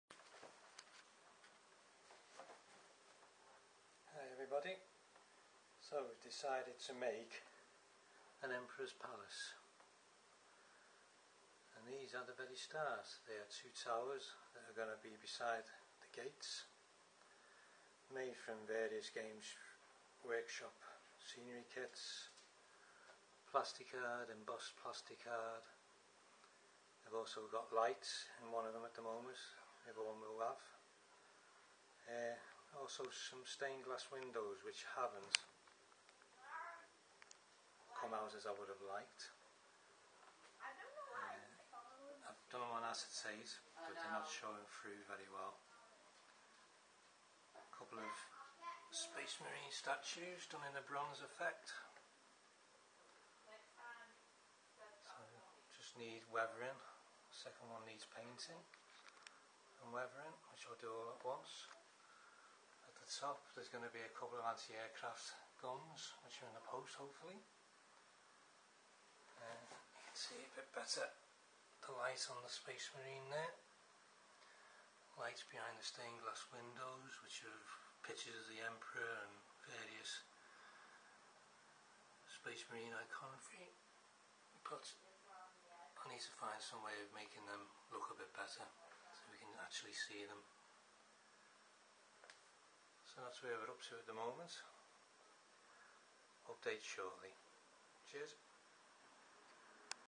warhammer 40k the emperors palace - update 1
so after seeing a fortress on here I decided I would like to do a emperors palace type fortress, lots of ornate features and very big, first up is the two large towers that will stand beside the 2 main gates, updates as I go along